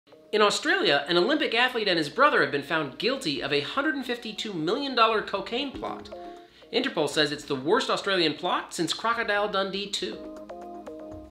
Australia Athlete Found Guilty of $152M Cocaine Plot (CLIP)
In Australia, an Olympic athlete and his brother have been found guilty of a $152M cocaine plot. Interpol says it's the worst Australian plot since Crocodile Dundee II.